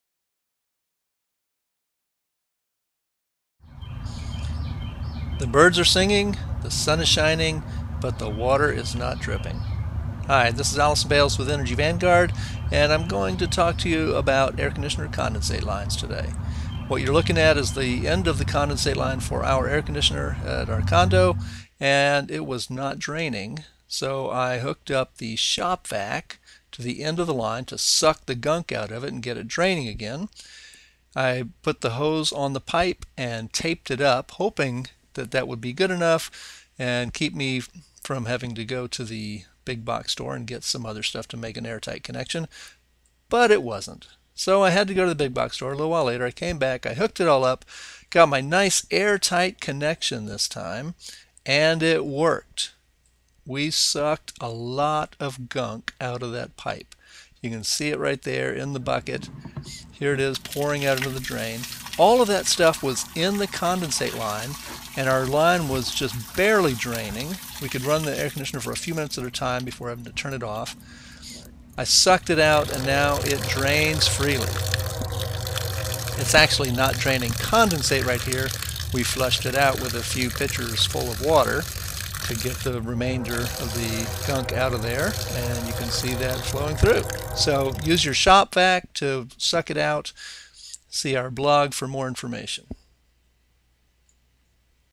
How to Clean Your Air Conditioner Condensate Line
Air conditioners dehumidify the air and that water has to go somewhere. That's what the condensate line is for, and sometimes it gets clogged. Here's one way to clean it out.

For more information on this topic, see the associated article in the Energy Vanguard Blog:

How to Clean Out Your Air Conditioner's Condensate Line